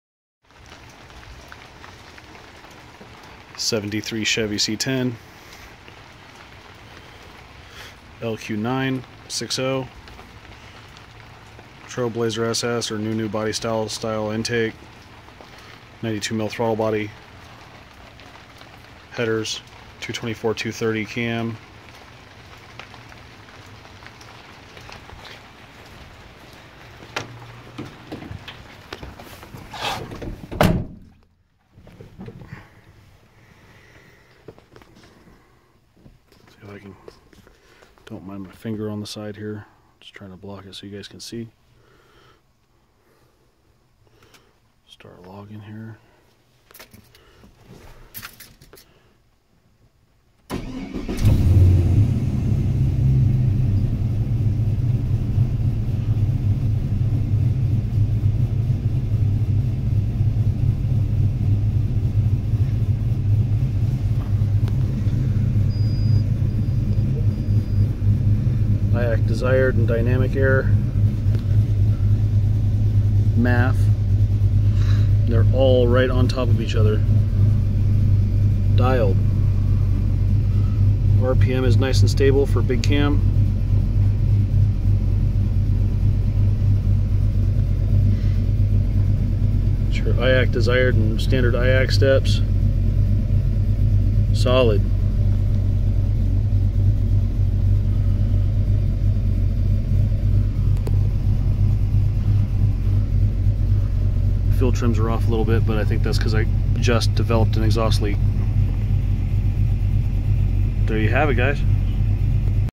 The rewards of IAC effective area tuning.
Performed rafig/rafpn or log maf in gear/pn, and fill "desired air" tables. Next, use rafig/rafpn to log against iac steps and figure out where IAC effective area needs adjustment and how much by logging maf vs steps to get an idea how many g/steps and use that to adjust your IAC effective area. Make sure idle learning is correct and adjusting quickly and precisely, and you have a very solid foundation to make your idle more stable.

I've also found ignition timing/short term fuel trims to cause surging from excessive adjustment as well.